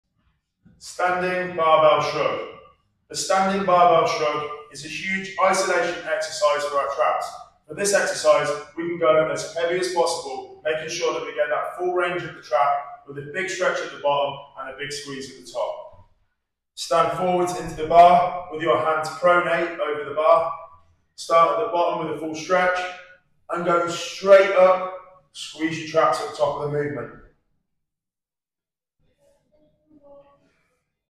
Rich Gozdecki Series Standing Barbell Shrug
Instructions:

1) Stand and hold a barbell with both hands in front of you at arms length, with your hands slightly wider than shoulder width apart.

2) Begin by raising your shoulders up as far as possible, pause, and then return to the starting position.